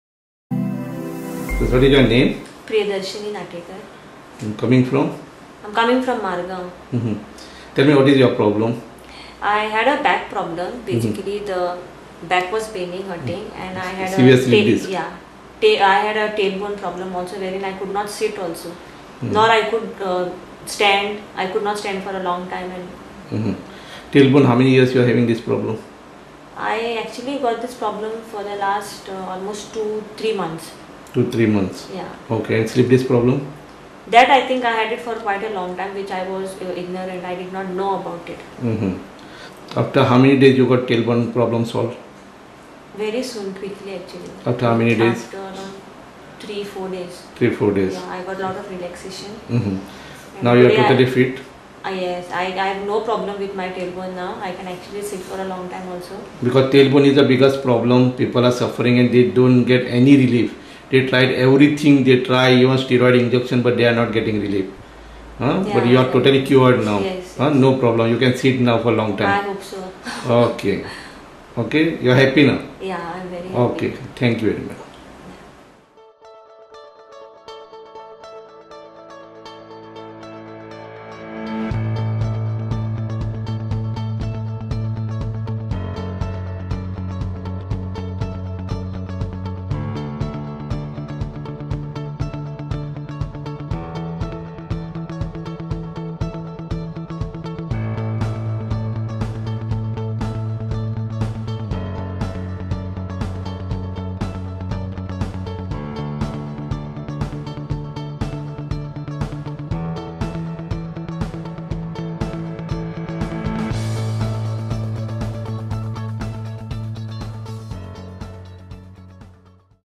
| Lumber Slip Disc | Tail Bone Pain| Treated with Acupuncture treatment At SARC-Goa Hospital English
Mrs. Priyadarshani Natekar was suffering from Lumber slipdisc for many years and tail bone pain for 2-3 months. She couldn't stand or sit due to pain.

She tried other treatments but had no effects.

Finally, she visited Dr. Prabhu at SARC-Goa hospital to perform, and with a few sessions of Acupuncture treatment, she was relieved completely from her suffering. With special Acupuncture treatment at SARC-Goa, she was successfully treated for her tail bone pain and back pain without any surgery or medicine.

With faith, Tiny needles can do wonders!

🙏🙏🙏Watch and please forward to all to reach everyone to help such patients who is suffering

Dr. M B Prabhu
SARCGOA ACUPUNCTURE RESEARCH CENTRE